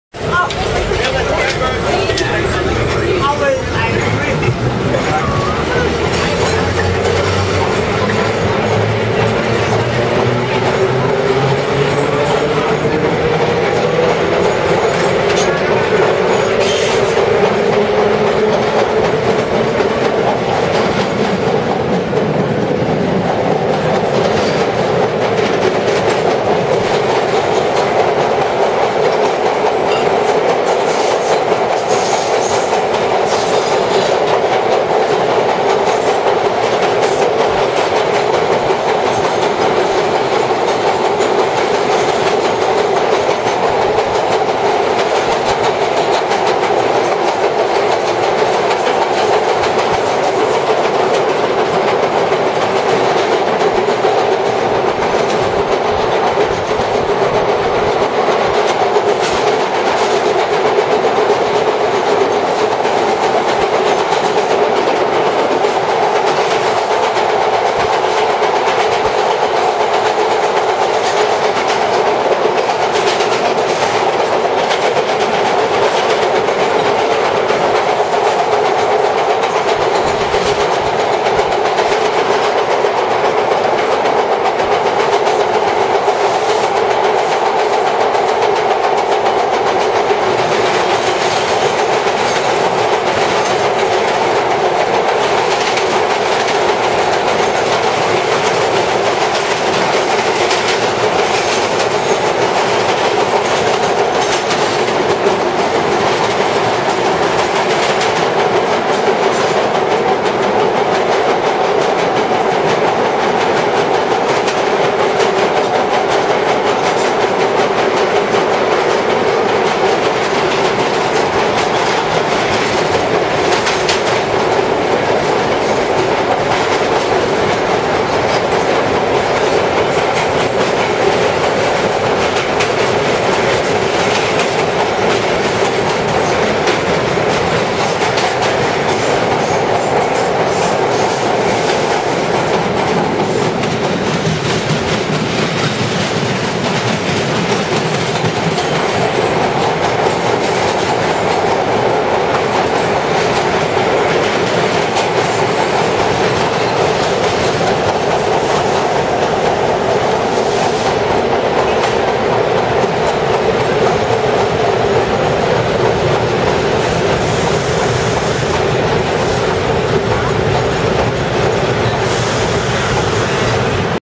MTA NYCT: R7A #1575 (M) Train (Queens Plaza-Bound)
its that time of year in 2013, the Arnines as a holiday train returns again but only for 4 sundays on the (M) Line, very excited. (1,8,15,22) 10:00AM - 5:00PM

Taken around the 12/15/13

(Flat Wheels)